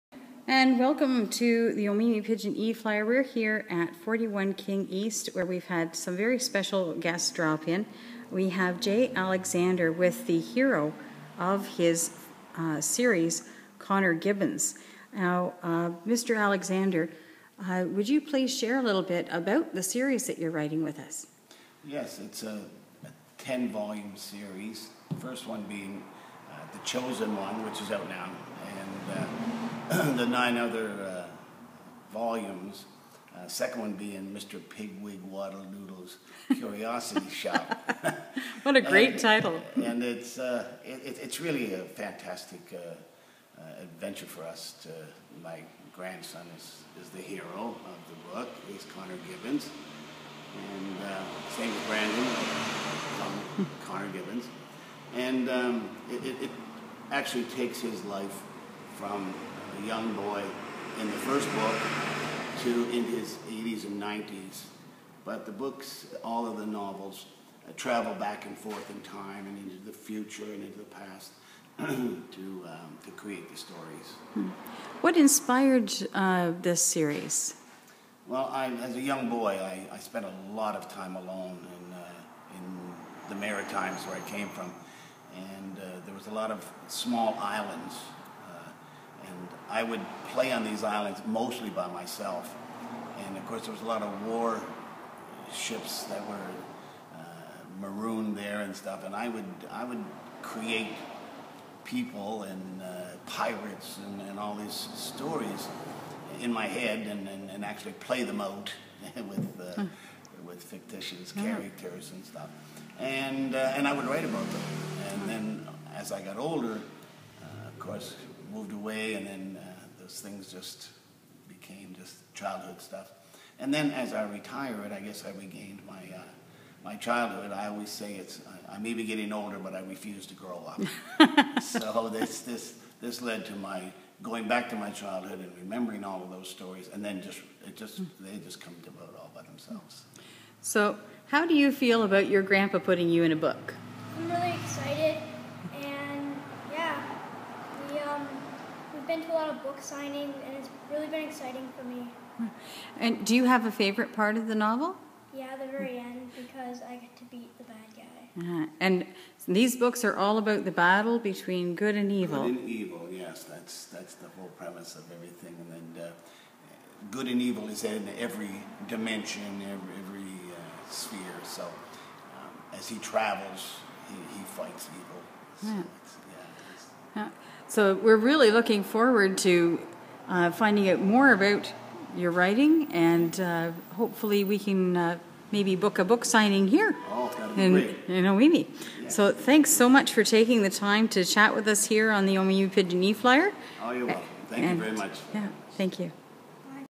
Christian Author Pens Fantasy Adventure Featuring Grandson
JAlexander ,and Connor Gibbons kindly took the time to share some insights about the first in this series.
Be sure to join their website,where you will find more information and some very special events !